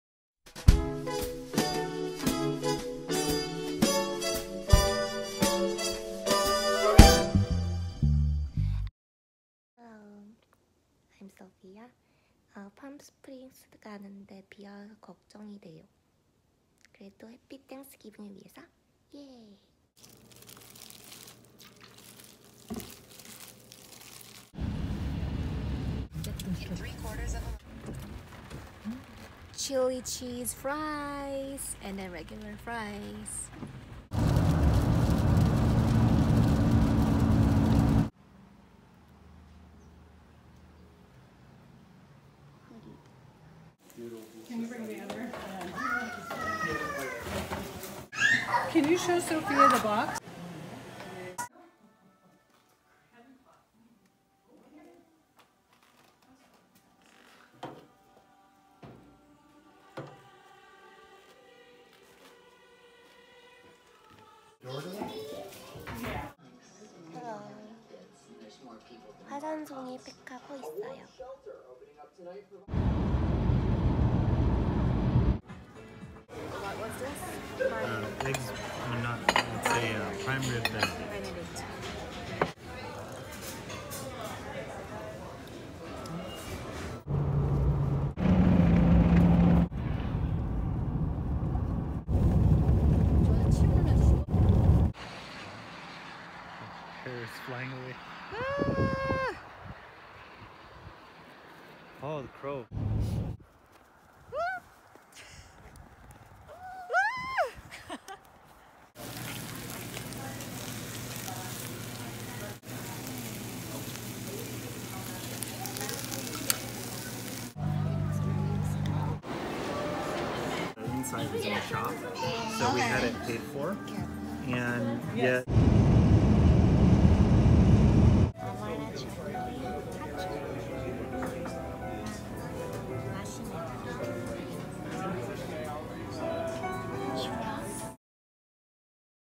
happy thanksgiving in Palm Springs
hi, guys
i visited Palm Springs and had a family dinner on thanksgiving,
and went to mount San Jacinto
AND IT WAS SNOWING!!!
And just so let you know, my video is very short,
but I hope you enjoy it